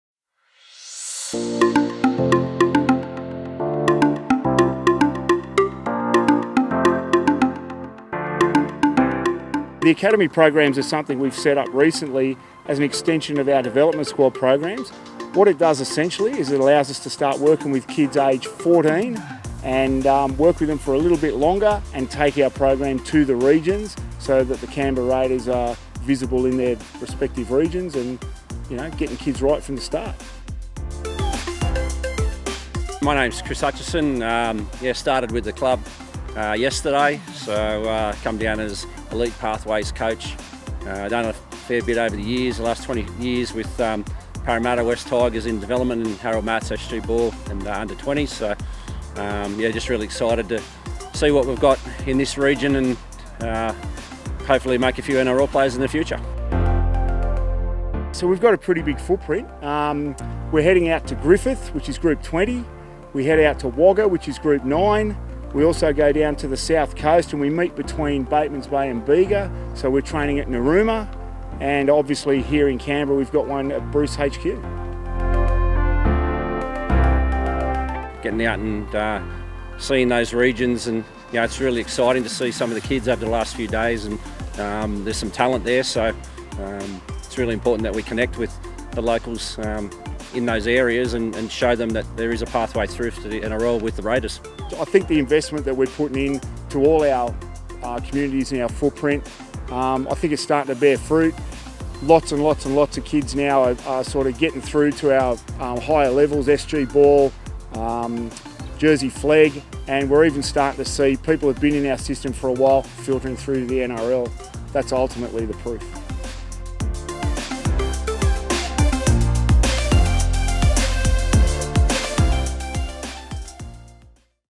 Raiders launch Academy Programs
The Canberra Raiders have increased their investment in their local regions, by establishing new Raiders Academies across the club’s regional footprint.